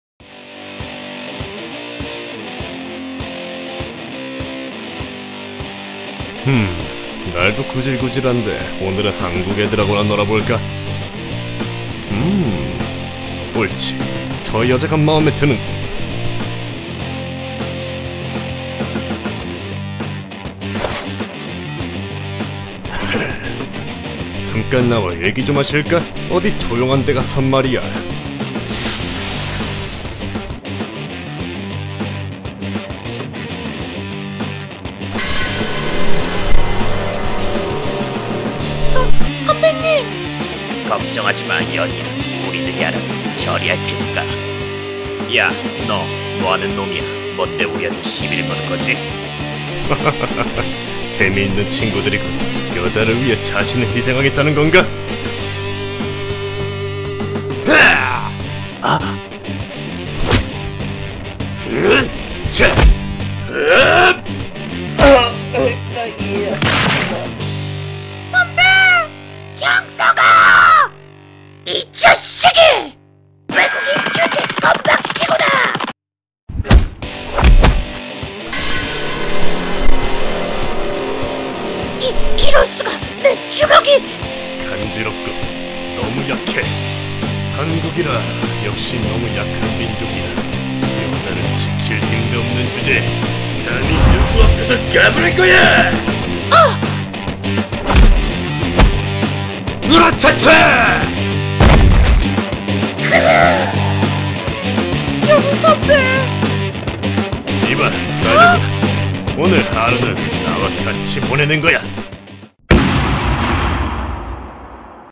[병맛더빙] 럭키짱에 나오는 미국인 클라스. 갈때까지 간 만화
병맛더빙 영상입니다. 한국남자 두명, 귀요미여자 역 ( 조마문 ) & 외국인클라스 역 ( 조마문크루 벤앤뱅츠 ) 로 둘이서 더빙 하였습니다.